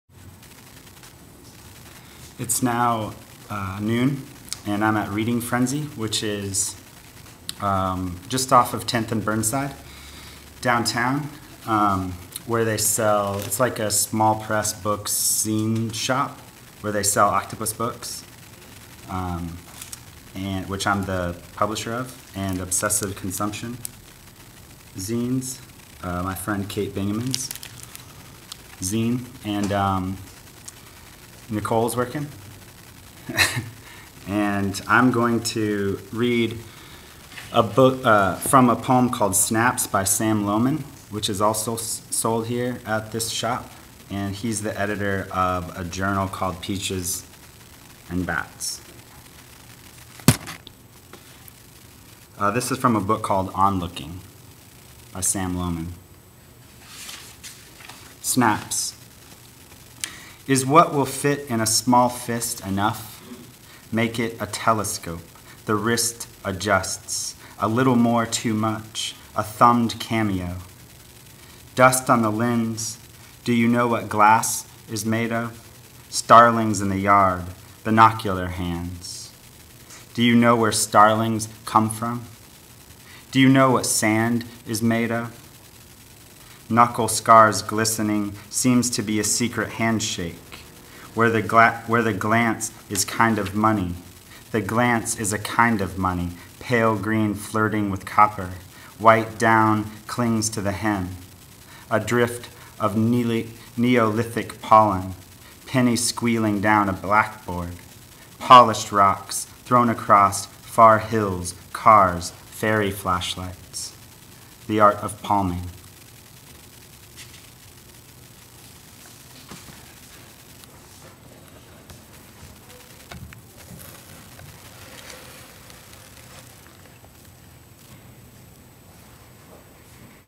12pm. Reading Frenzy.
from "Snaps" by Sam Lohmann from his book Onlooking (Airfoil 2009).